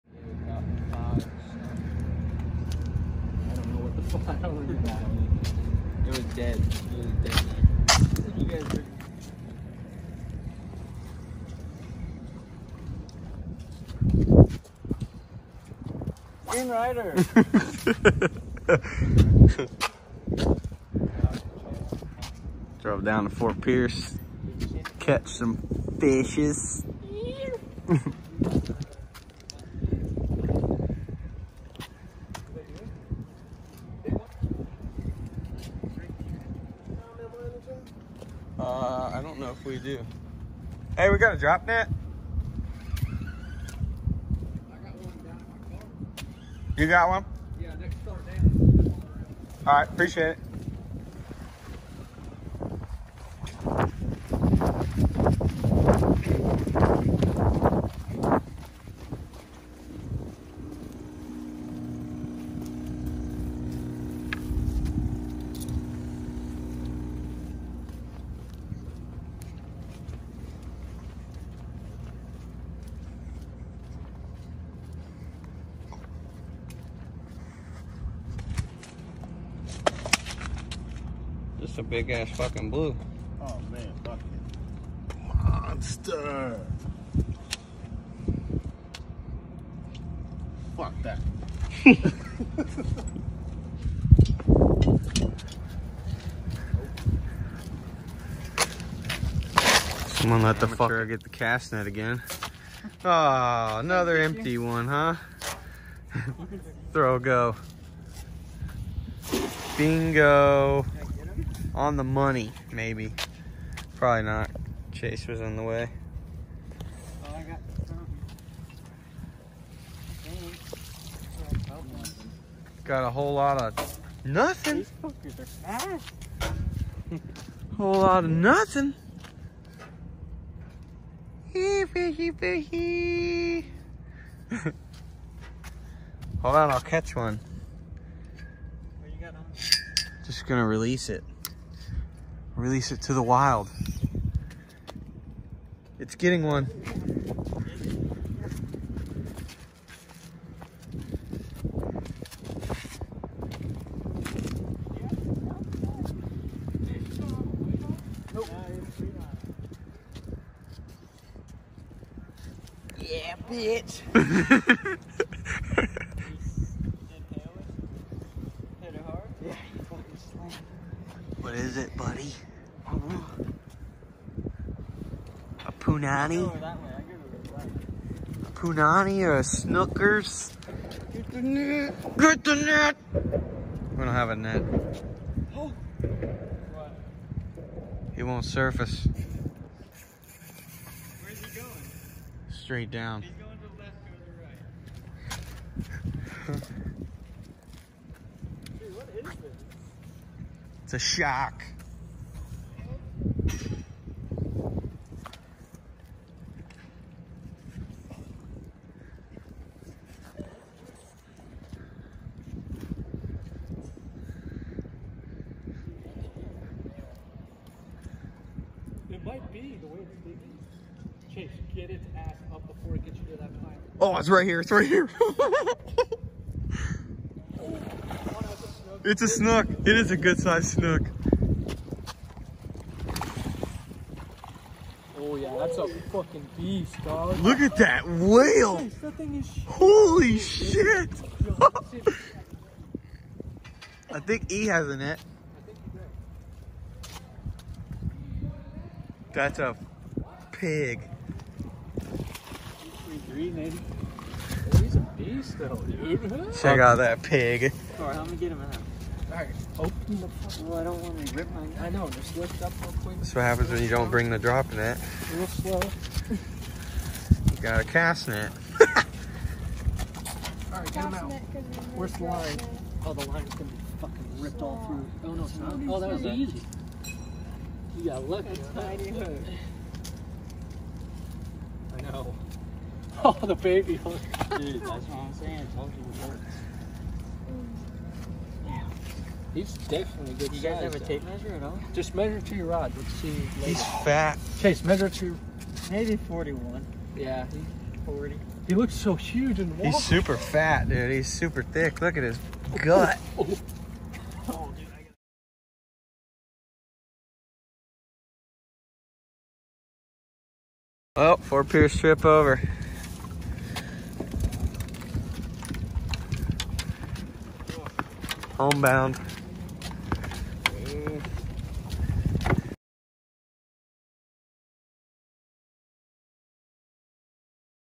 Late night Monster snook Ft. pierce
Took a late night trip to do some fishing with some buddy’s in Ft. pierce Florida caught a few small fish and a nice size blue fish and a monster Fat 40” snook